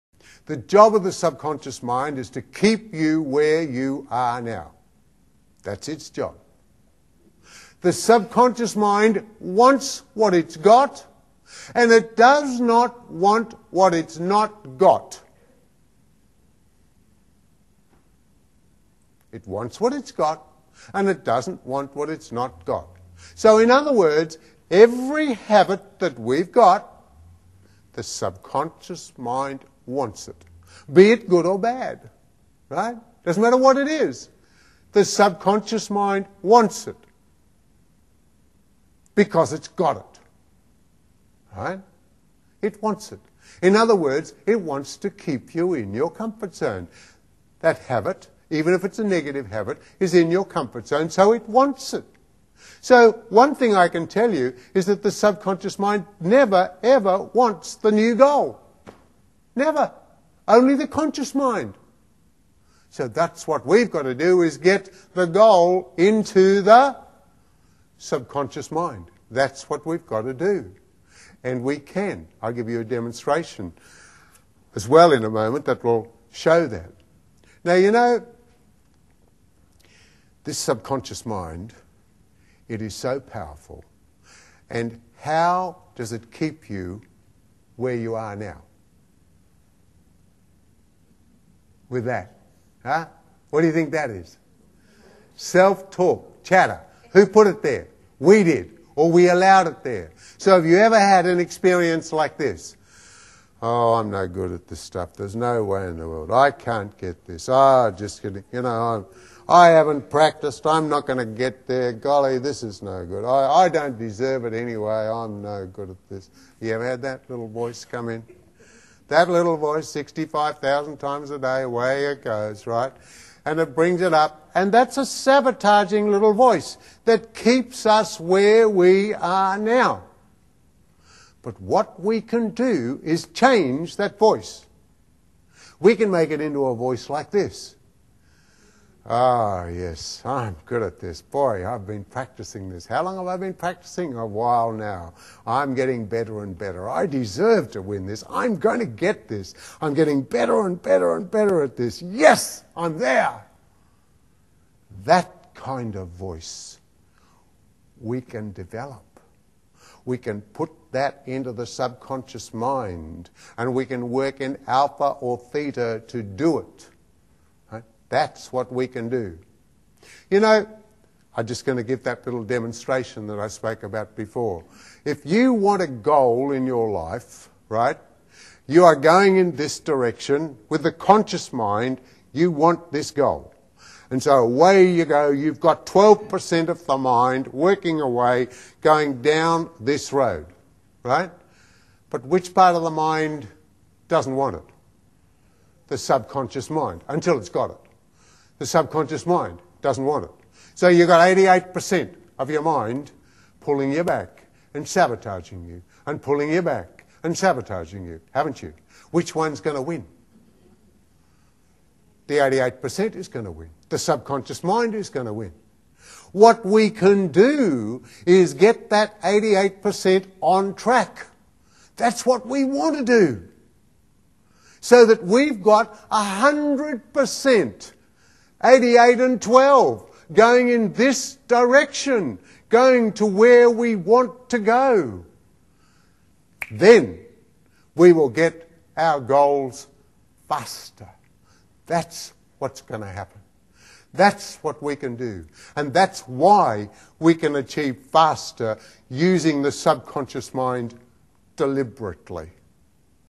CALM - The Job of the Subconscious Mind
This explains the job of the subconscious mind and the role of sabotage in keeping us where we are now.